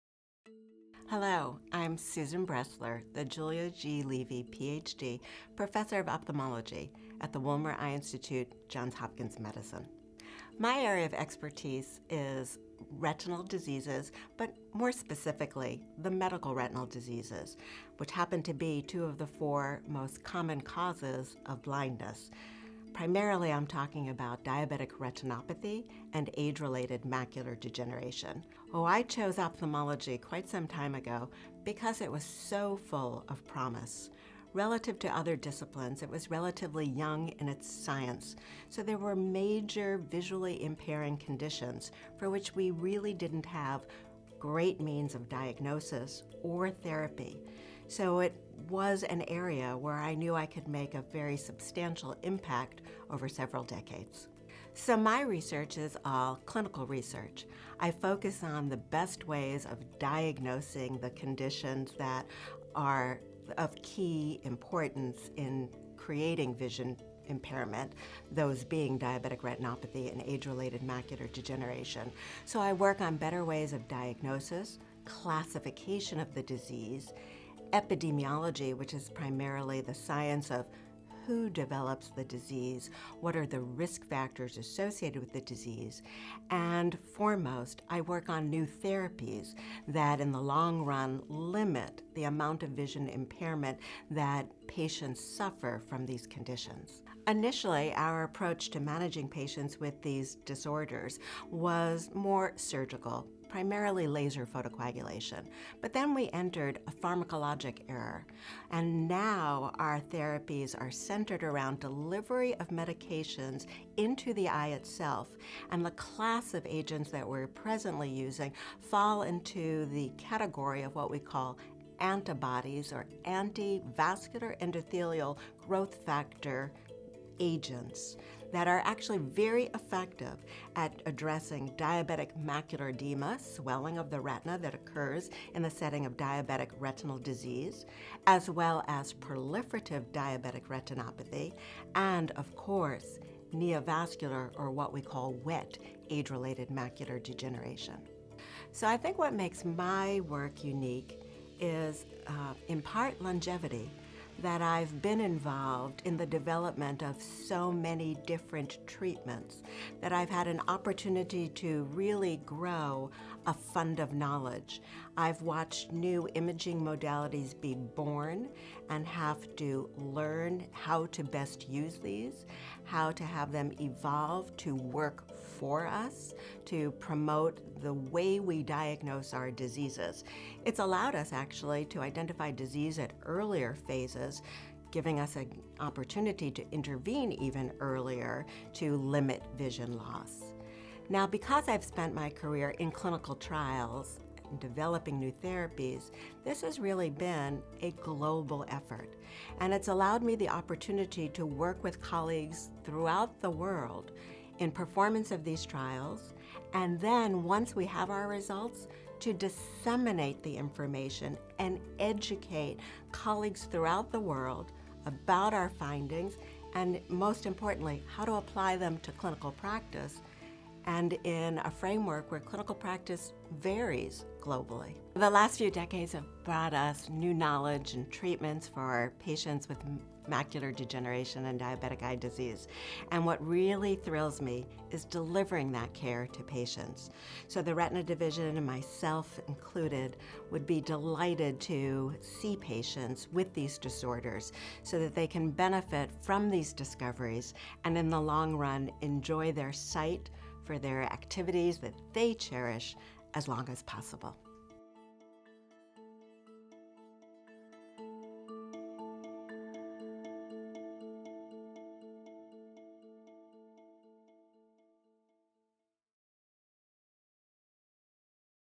Dr. Susan B. Bressler | Retina Specialist
Dr. Susan Bressler is the Julia G. Levy, Ph.D. Professor of Ophthalmology at the Wilmer Eye Institute. She is a board-certified ophthalmologist and has subspecialty training in medical retinal disorders, vitreoretinal disease, and retinal surgery. Her main research interest has been collaborative efforts in clinical trials - serving as principal investigator of an image reading center that has served as a central unit for many clinical trials and epidemiologic investigations, as Vice Chair of the Diabetic Retinopathy Clinical Research Network and as principal investigator of a participating clinical center in several major clinical trials. Most studies have a specific emphasis on the treatment of both non-neovascular and neovascular age-related macular degeneration and all aspects of diabetic retinopathy.

Dr. Susan Bressler has a large national and international referral practice. Also, she has published 223 peer-reviewed articles and 55 book chapters. Editorial Board positions have included American Journal of Ophthalmology, Survey of Ophthalmology, Retina, EyeNet Magazine, Health After 50: The Johns Hopkins Medical Letter and the Wilmer Retina Update.